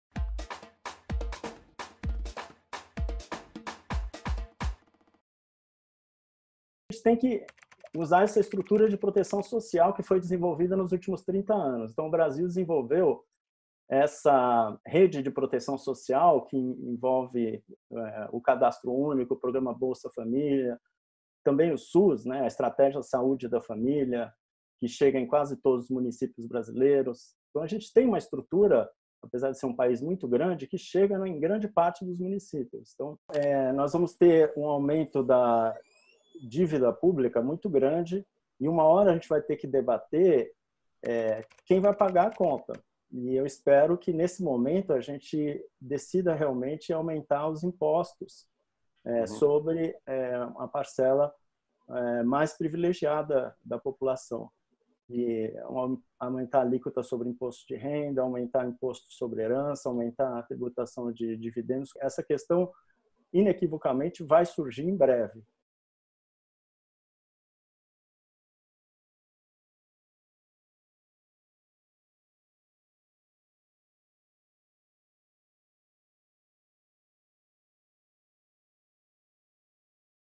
BAS Minutes #9 | Post-coronavirus Economic Crisis
Naércio Menezes, member of the Brazilian Academy of Sciences, talks about the solutions to the post-coronavirus economic crisis.